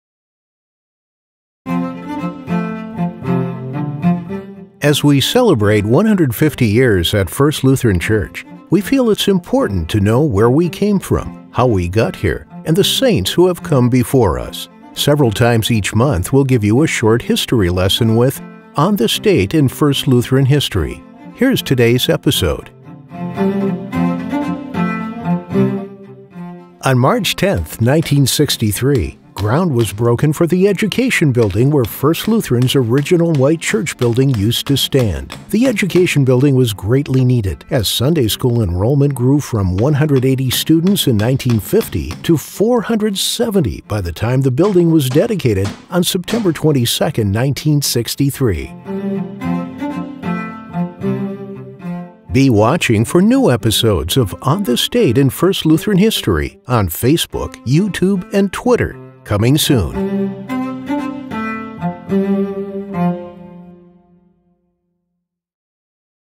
On This Date in First Lutheran History • Ep. 10
As we celebrate 150 years at First Lutheran Church, we feel it’s important to know where we came from, how we got here and the saints who have come before us.

In this episode we revisit 1963 and the construction of the Education Building.